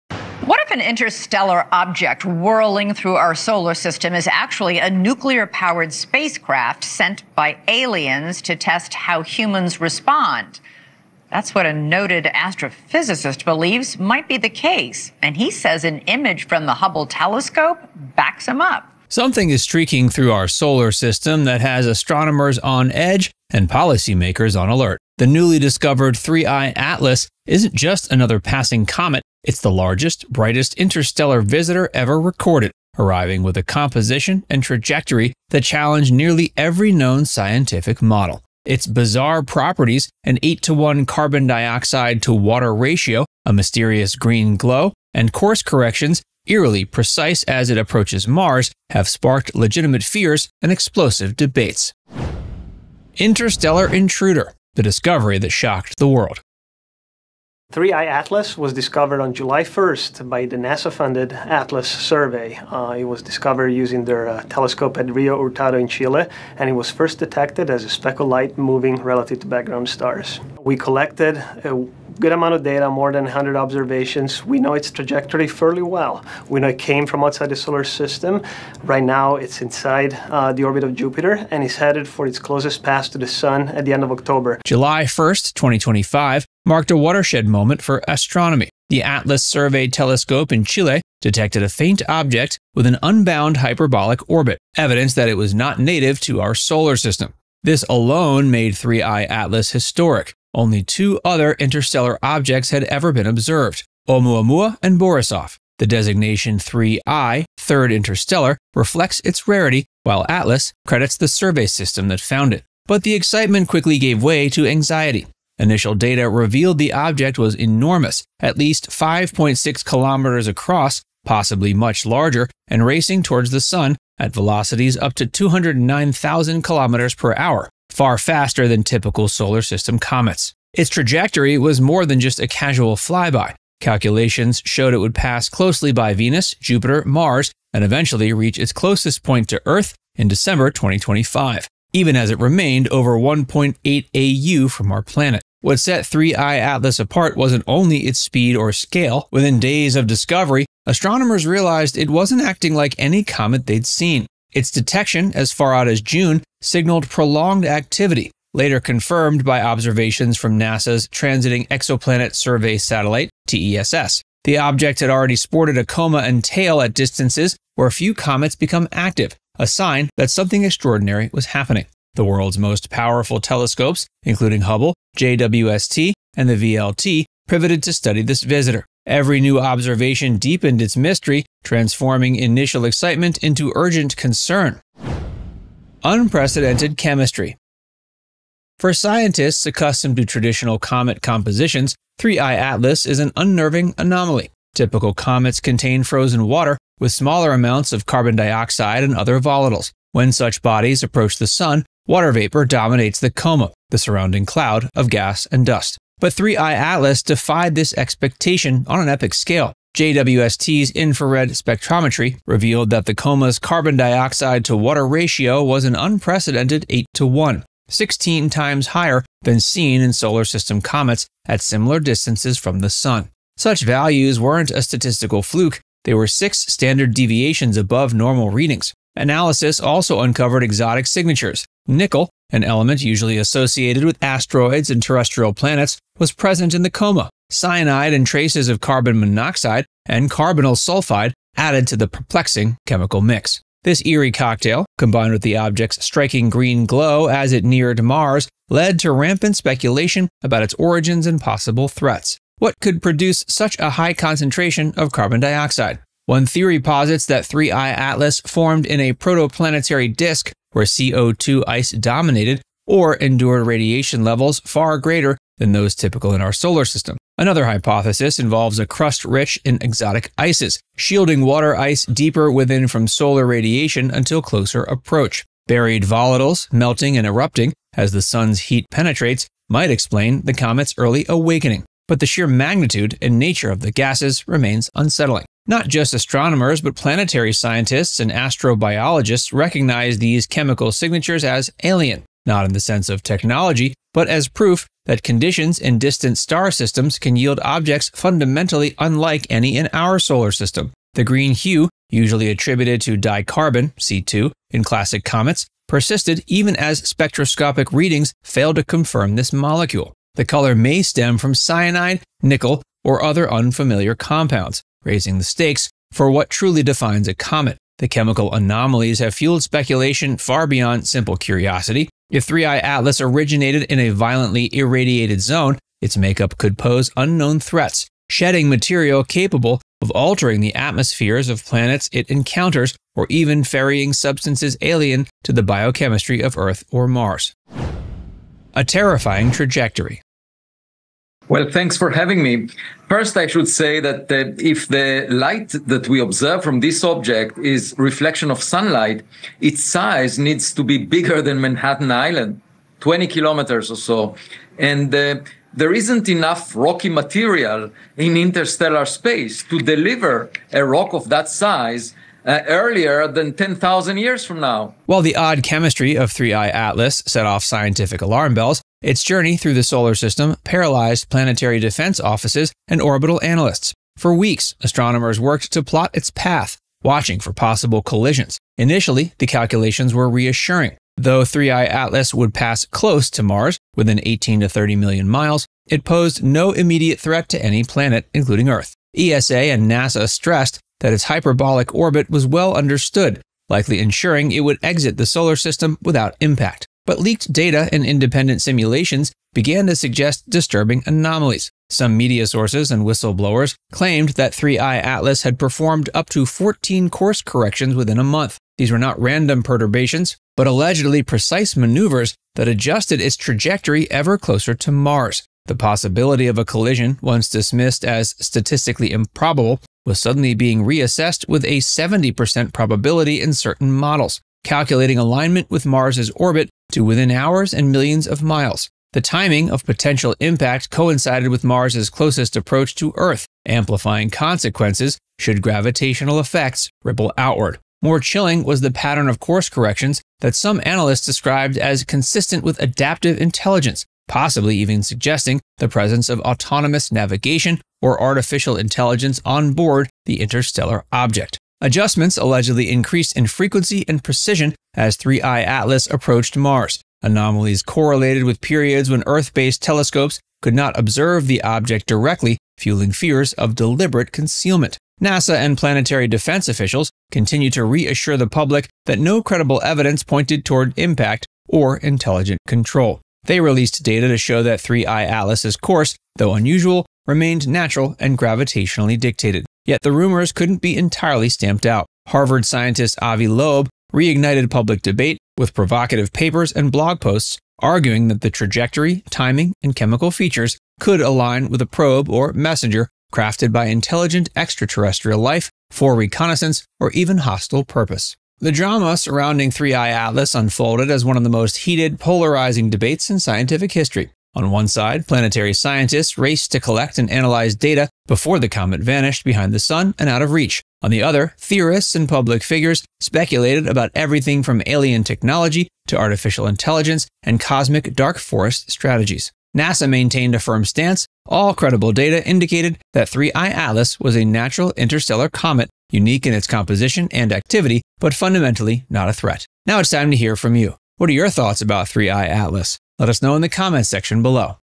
New 31/ATLAS Discovery Has NASA & Scientists Terrified
A celestial drama is unfolding above our heads, and the stakes couldn't be higher. "New 31/ATLAS Discovery Has NASA & Scientists Terrified" delves into the astonishing true story captivating the world's leading astronomers and institutions. Rarely does a single cosmic object pull the collective gaze of every major space agency, but 3I/ATLAS—a vast, fast-moving interstellar visitor with a chemistry and path unlike anything seen before—has triggered a chain reaction of scientific urgency and public curiosity.

00:00 - Intro
00:53 - Interstellar Intruder—The Discovery That Shocked the World
03:05 - Unprecedented Chemistry—A Celestial Recipe for Alarm
05:38 - A Terrifying Trajectory—Precision, Course Corrections, and Impact Scenarios
08:49 - Outro

What makes 3I/ATLAS so terrifying? It's not just its alien origins, but the shocking revelations found in its wake. As the largest, brightest interstellar body ever recorded, its approach is punctuated by inexplicable chemical signatures: an astonishing eightfold excess of carbon dioxide compared to water, unexpected traces of nickel, cyanide, and a mysterious green glow now visible to even amateur sky-watchers. Each discovery shatters previous scientific expectations, hinting at conditions and processes that lie far beyond the reach or imagination of traditional astronomy. And just when things couldn't get more intense, observations begin to document erratic course corrections as the object zeroes in on Mars—provoking claims of advanced guidance, or something even more elaborate.

Will 3I/ATLAS merely pass through, changing our models of solar system formation forever, or could its presence mean far more—potentially bringing cosmic contaminants, interstellar threats, or proof that we are not alone? NASA, ESA, and the world's top planetary defense agencies have mobilized, tracking the object's every move and sharing real-time discoveries with the public. Controversy swirls as renowned figures debate whether this visitor is a natural wanderer—or an engineered probe, sent with purpose by forces unknown.

If you crave breakthrough science, cosmic mysteries, and the thrill of discovery, this video is your gateway to understanding both the facts and the fears surrounding 3I/ATLAS.